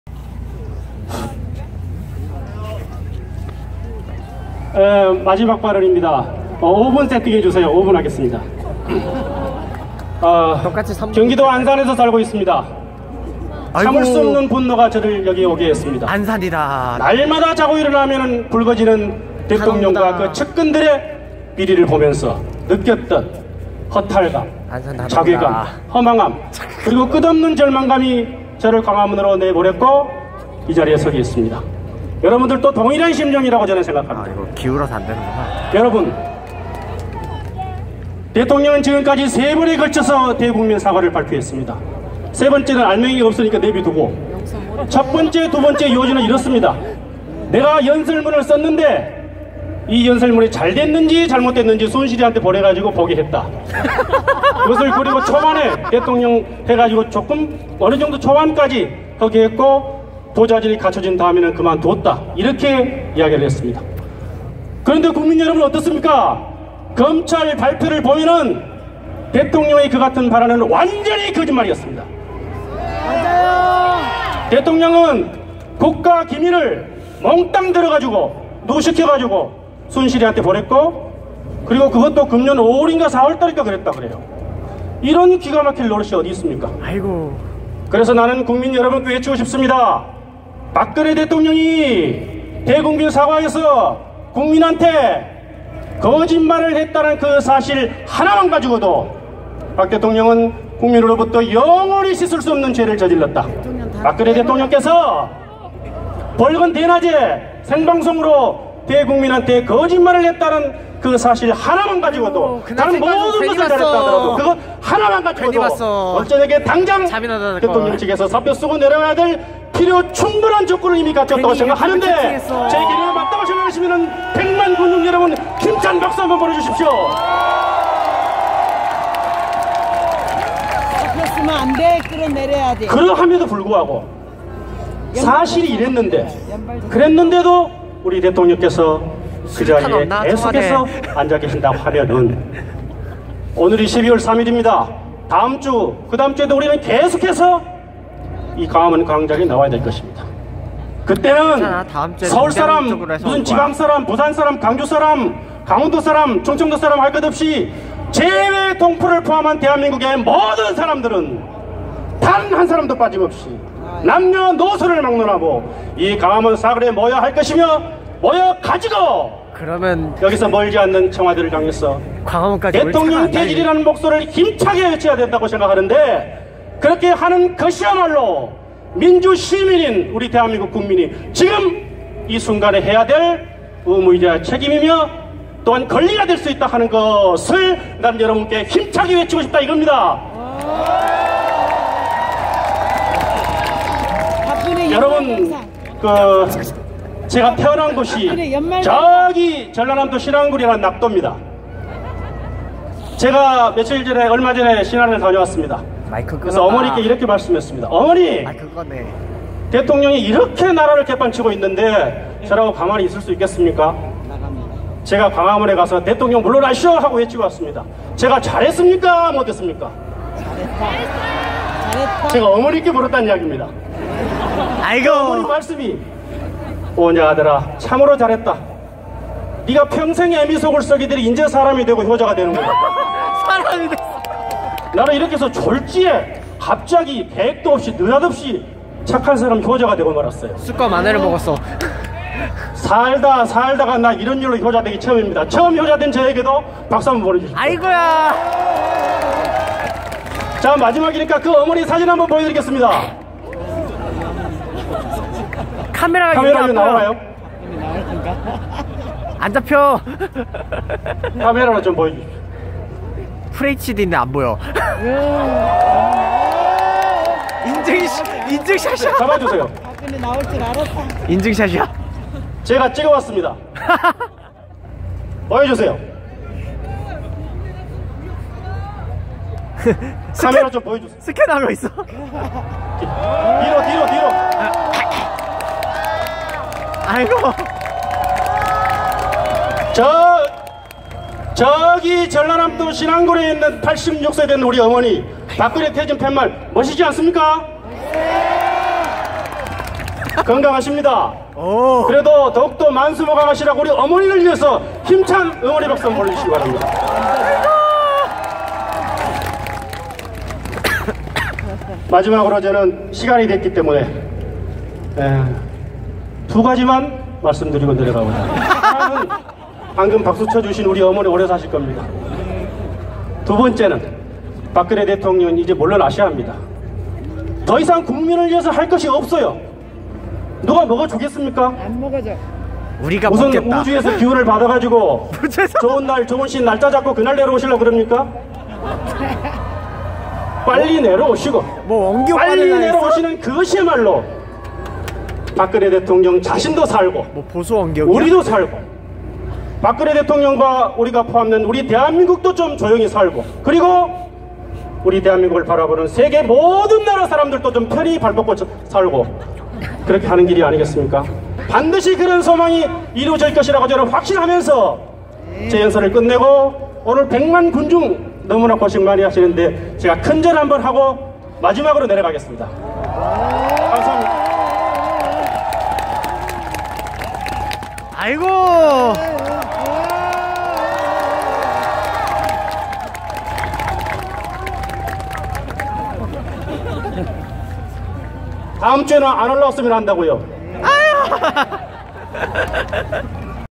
광화문에 와서 효자된 신안 아저씨 자유 발언 @ 광화문광장 2016.12.03
이 분 말은 좀 하신 듯. 정치계에 이런 사람이... 없죠. 아쉬울 뿐.